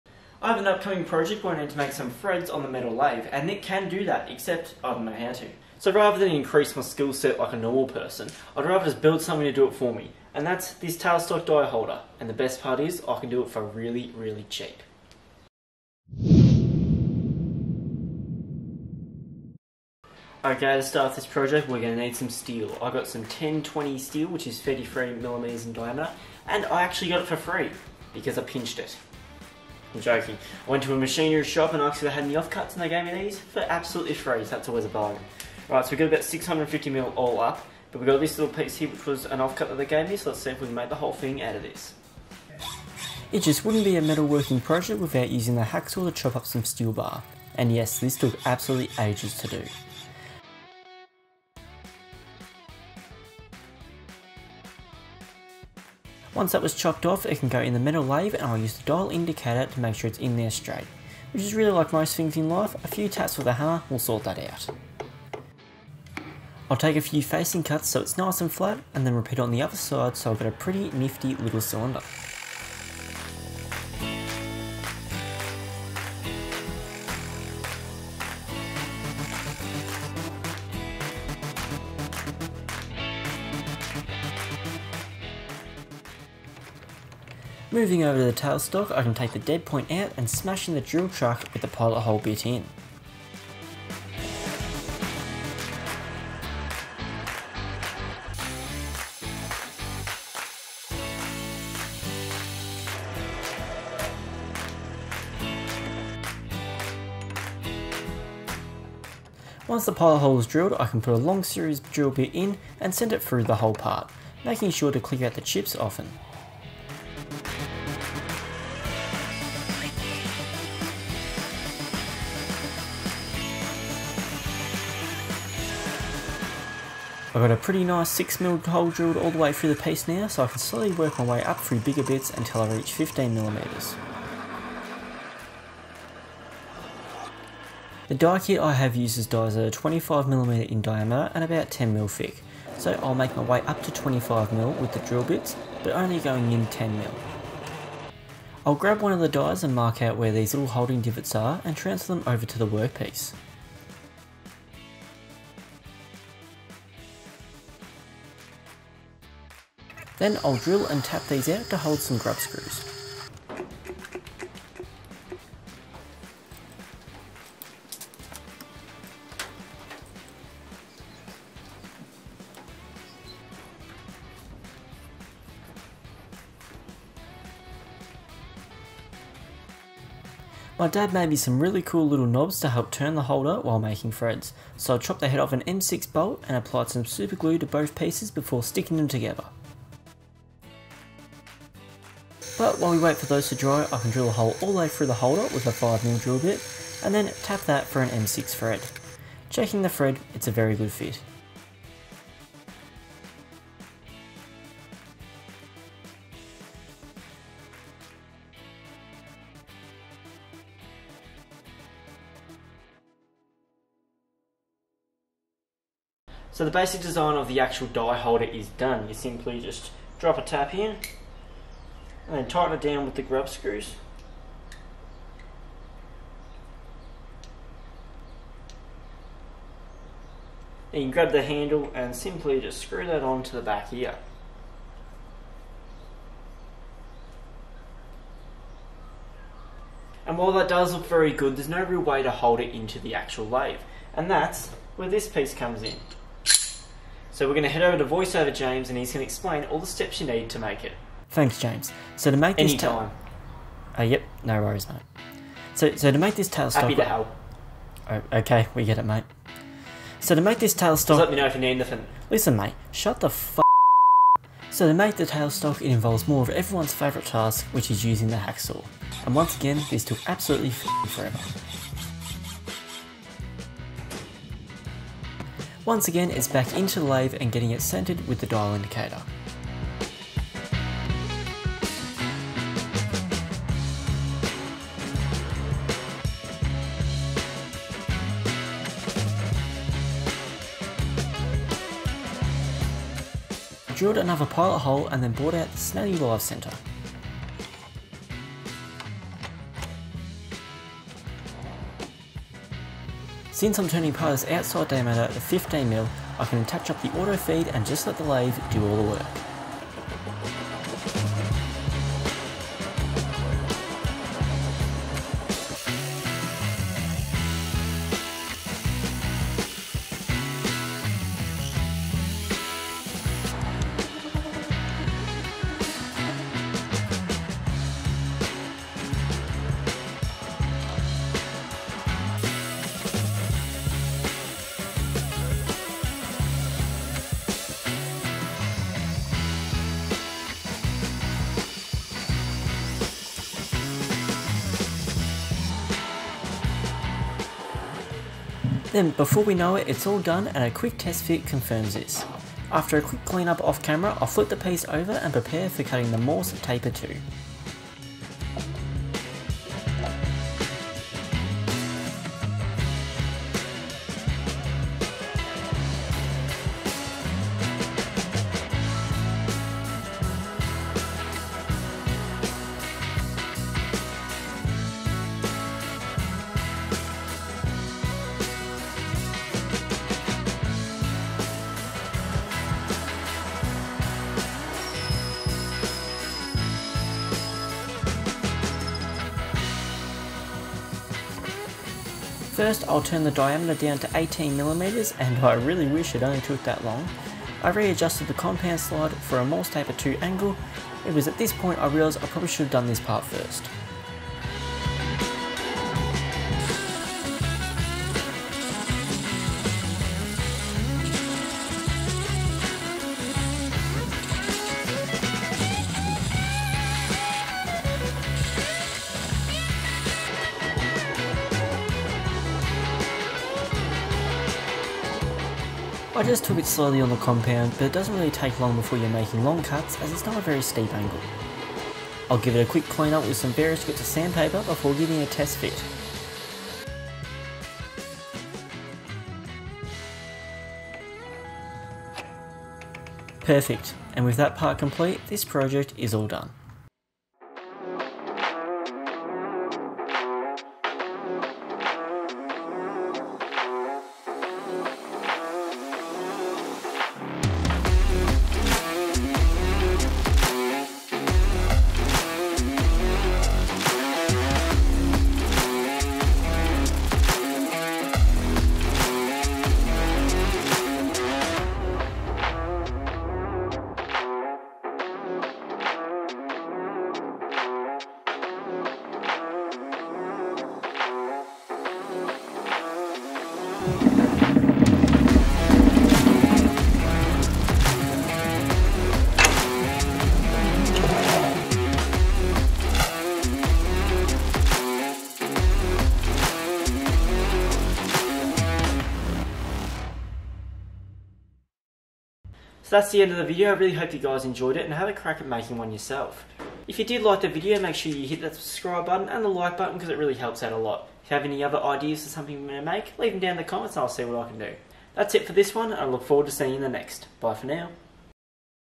Machining a Tailstock Die Holder for FREE!
Making threads on the metal lathe requires certain cutting tools so instead i made a Tailstock Die Holder to make those threads for me. But the best part is, they do not cost much to make at all.

Chapters:
0:00 - Intro
0:20 - Making the Die Holder
4:00 - Interrupting James
4:24 - Making the Tail Stock Mount
7:26 - Final Test / Outro